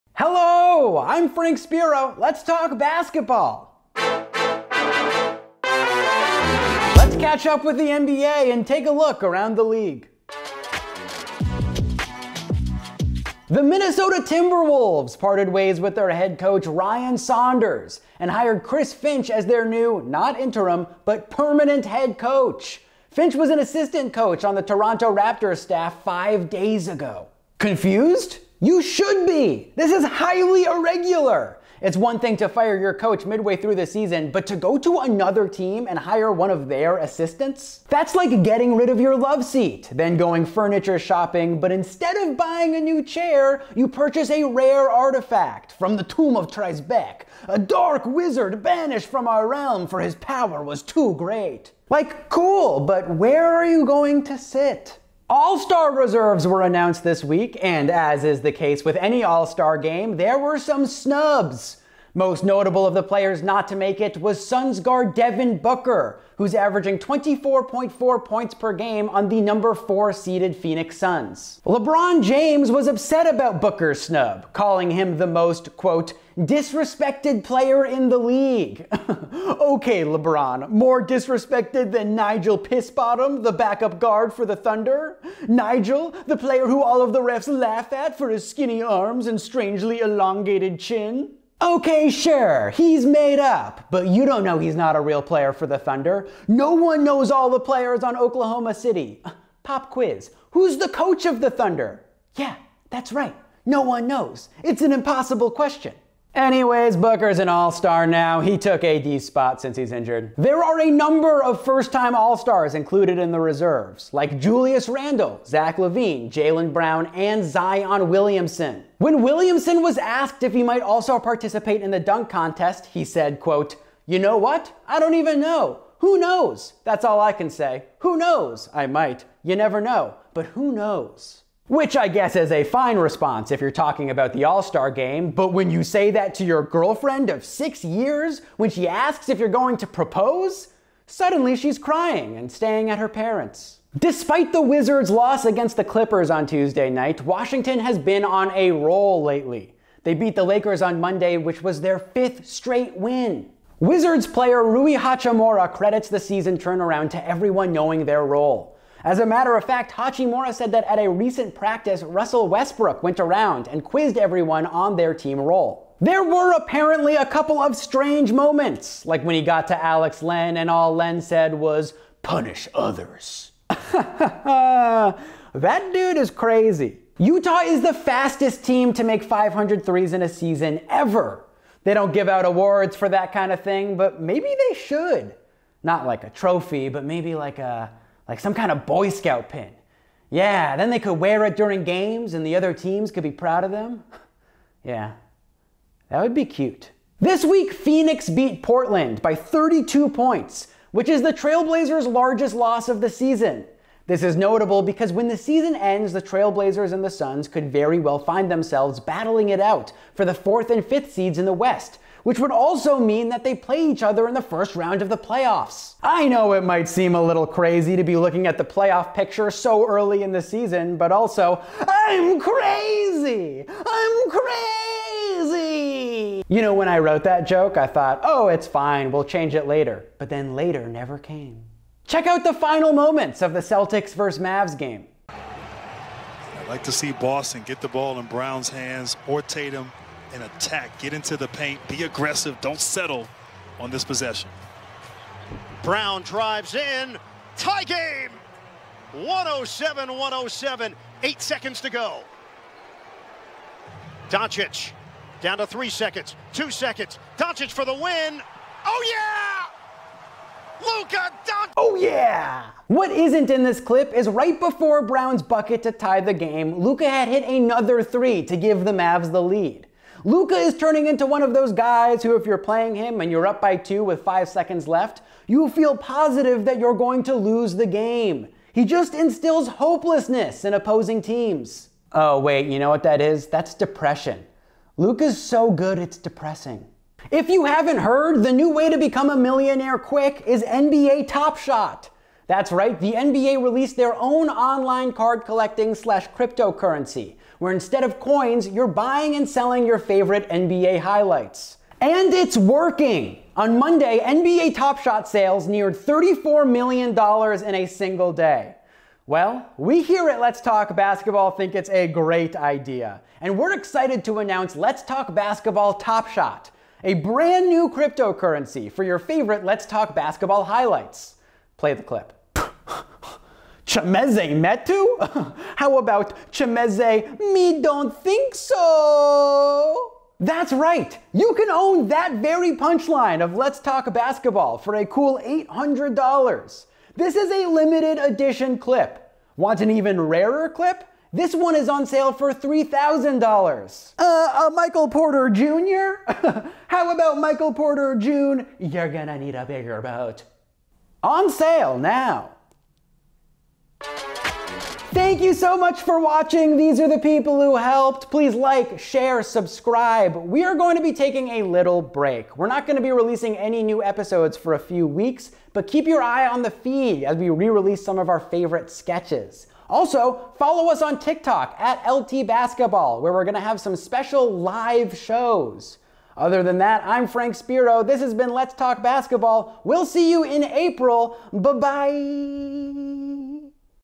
Around the League 2.26.21 -- Will Zion compete in the dunk contest? Who were the All Star snubs?
Frank takes a look around the NBA on 2.26.20. Will the Trailblazers and Suns meet in the playoffs? What's with this new NBA Topshot craze?